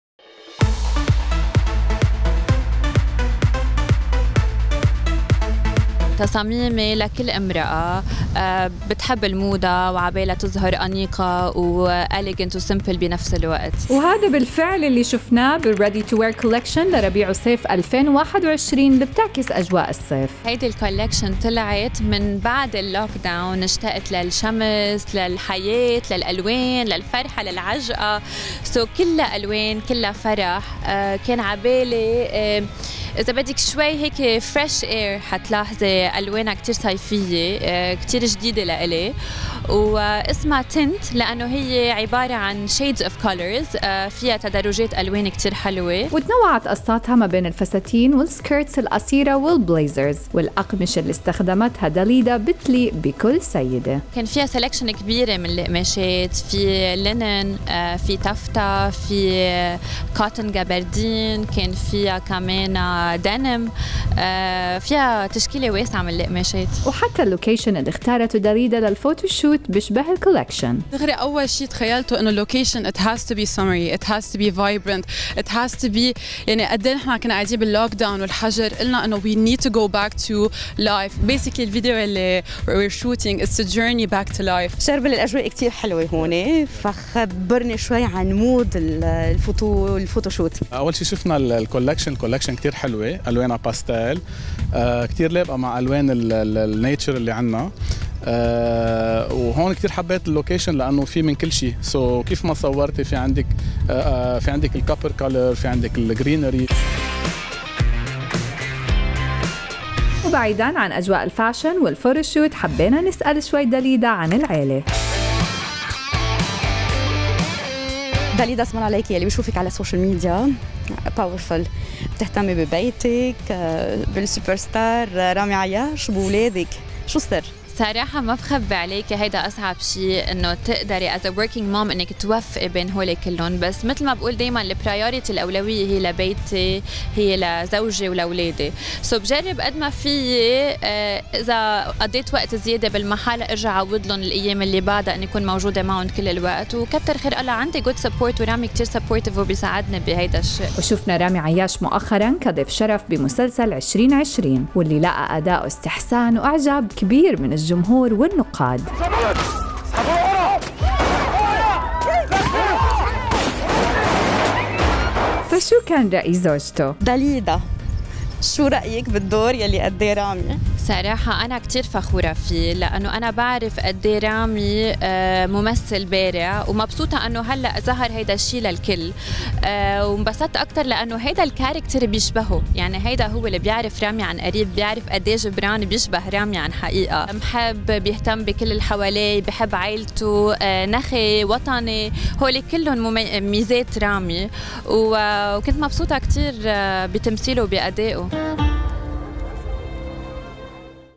داليدا عياش تعكس أجواء الصيف بمجموعة جديدة تناسب كل سيدة
وتتحدث عن عائلتها ورأيها في ظهور زوجها رامي عياش بمسلسل عشرين عشرين